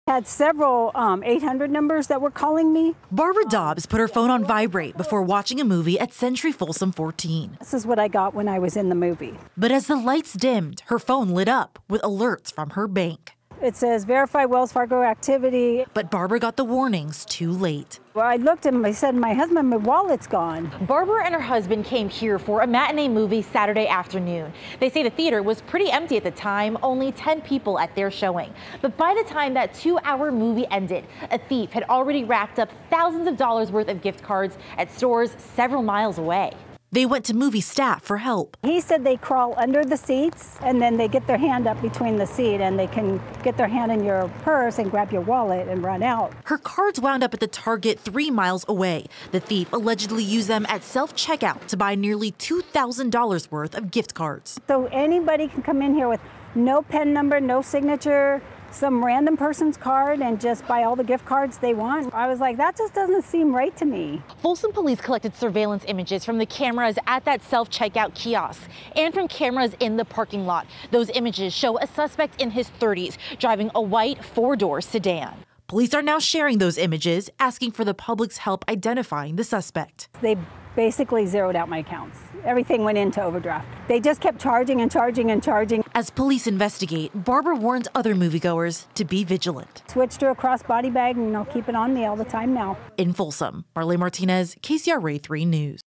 Woman says her wallet was stolen while watching movie at Folsom theater
Barbara Dobbs reached out to Folsom police after she said someone stole her wallet by crawling underneath movie theater seats during a matinee showing at Century Folsom 14.  Before the movie was even finished Saturday afternoon, the thief allegedly took her debit and credit cards and racked up nearly $2,000 worth of charges, Dobbs said. Get the full story in the video above.